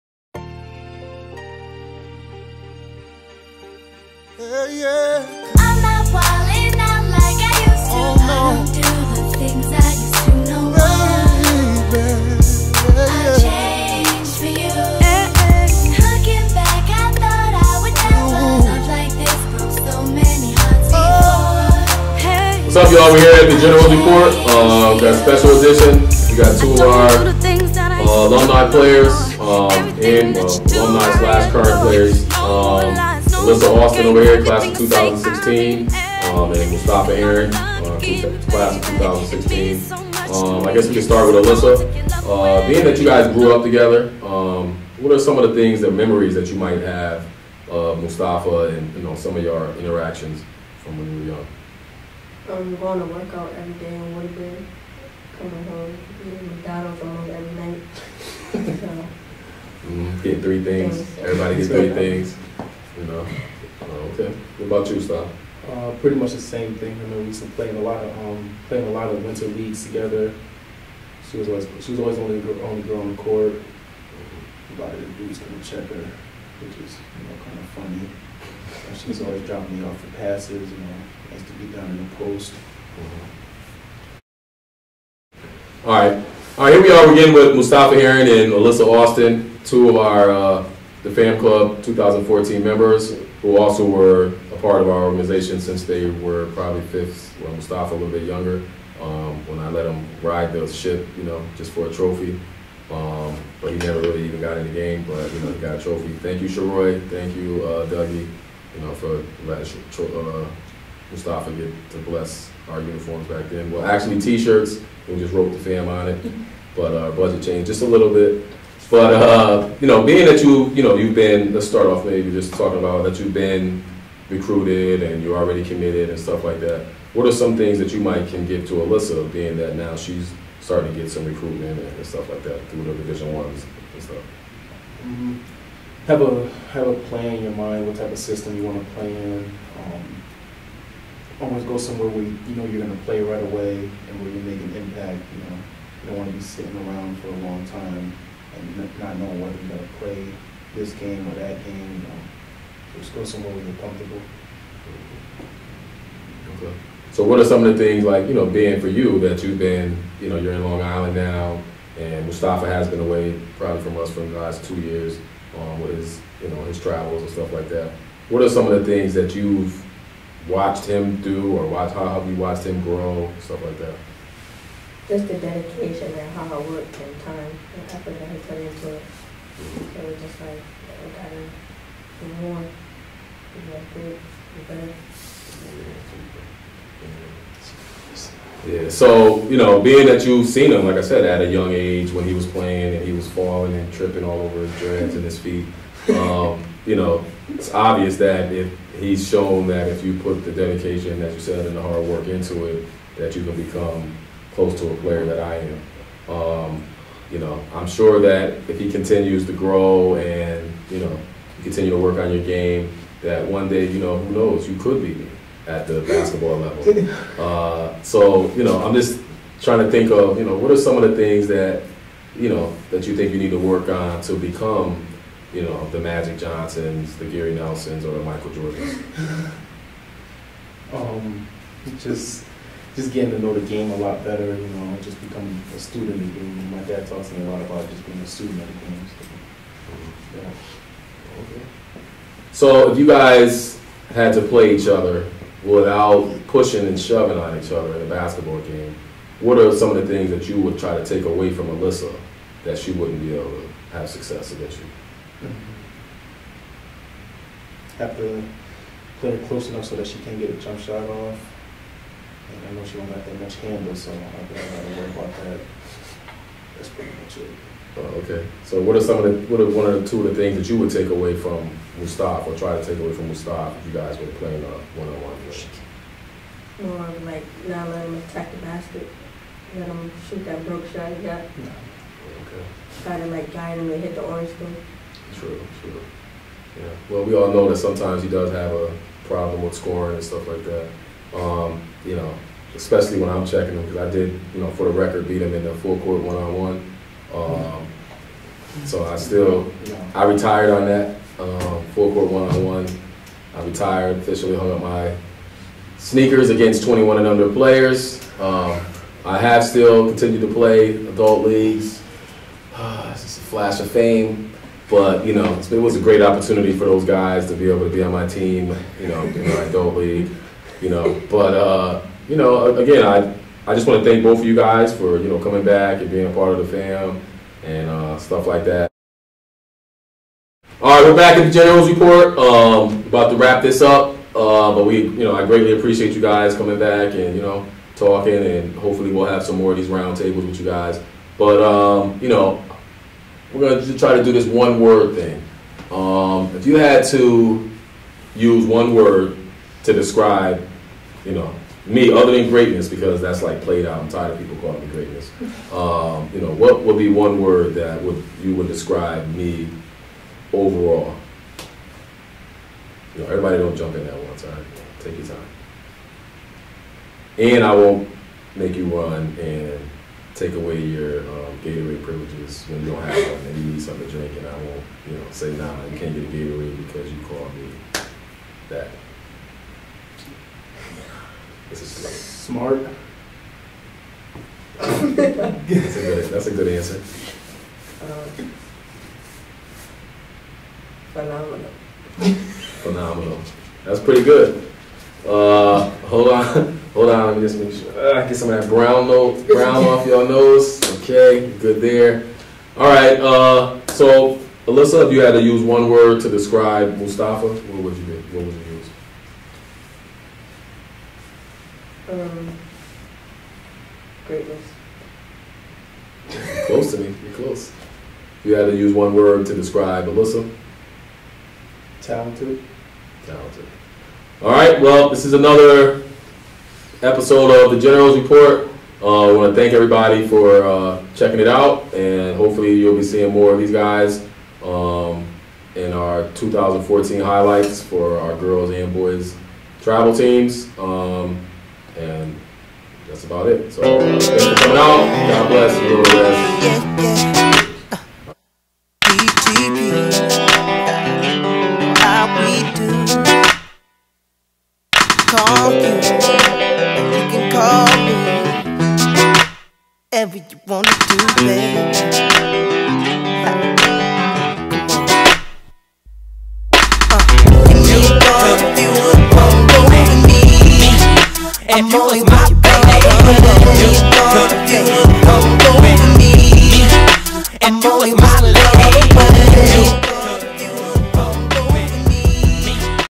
CT'S Alissa Alston(pg/2016) and mustapha heron(6'6/pg/2016)
DA FAM Club 2 Top players talk about their experiences growing up together in Da FAM Club in new haven,CT... YOUTUBE CHANNEL "DA FAM CLUB TV"